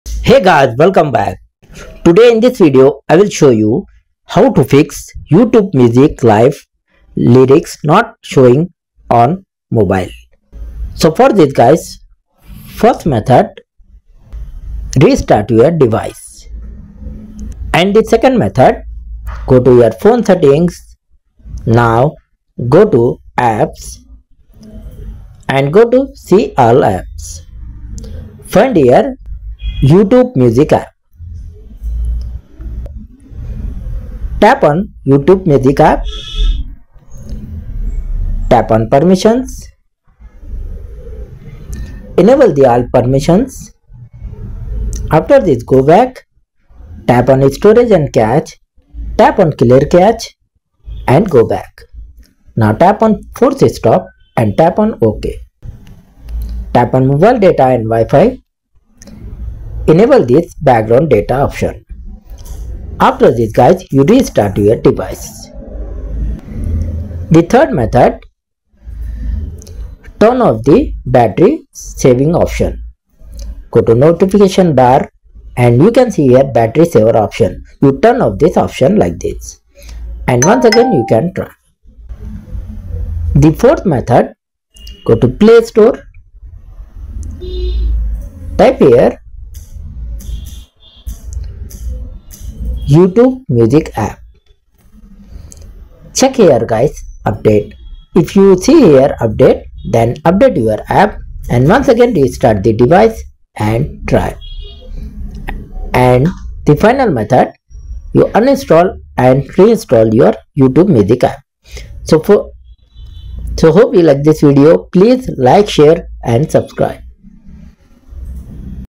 How To Fix YouTube Music Live Lyrics Not Showing
In this tutorial I will show you, How To Fix YouTube Music Live Lyrics Not Showing.